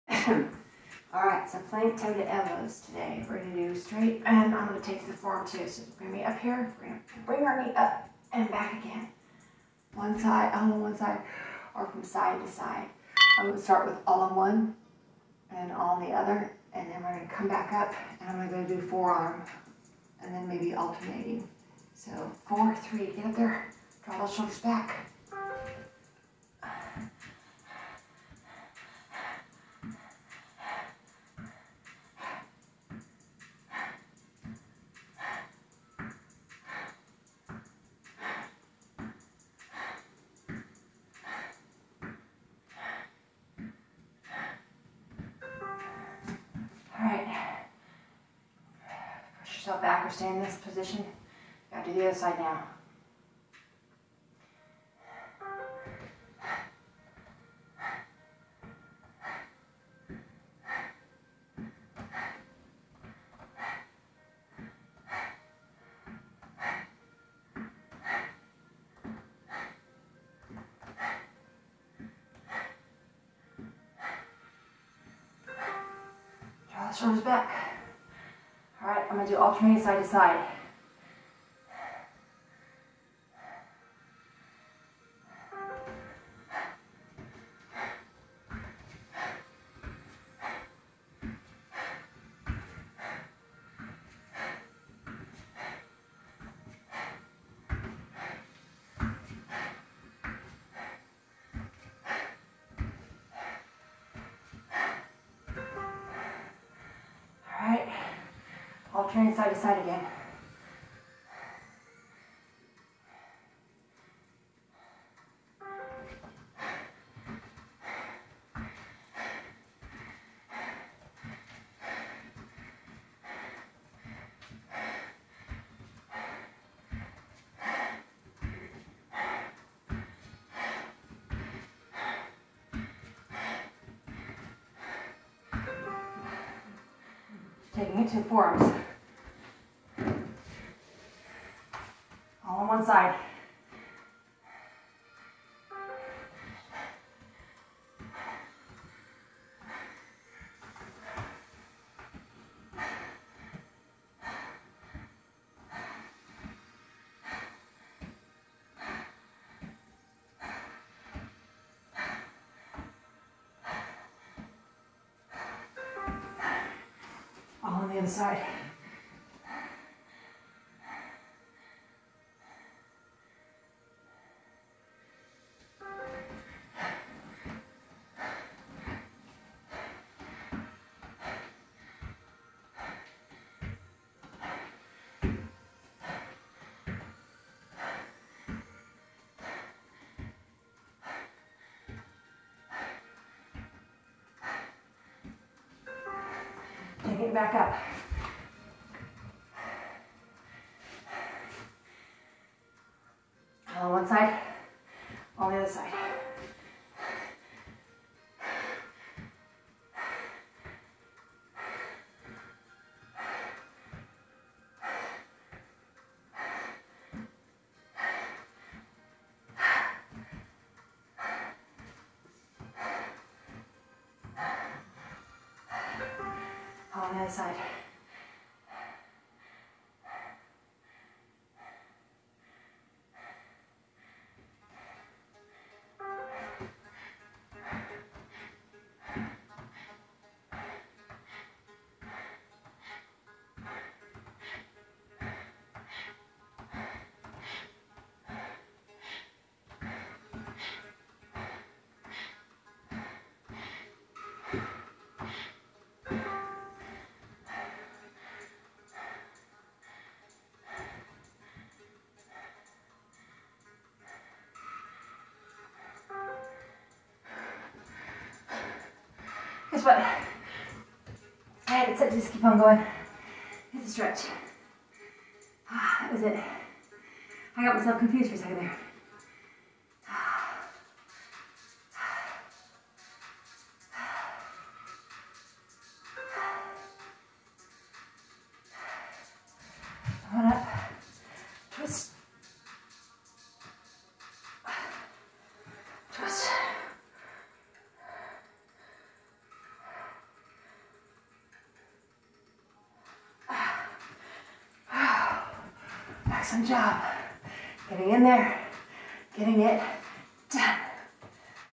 Day 10: Plank Straight Leg Toe to Elbow
I am thankful for my son Joey and to see my father and husband he has become.

Start in the top of a push up, regular plank or on your Forearms, in a wide stance.

With your leg straight bring your foot out towards the side of your body, towards your elbow.

 Alternate from side to side for 20 seconds, 10 seconds recovery and repeat 8x.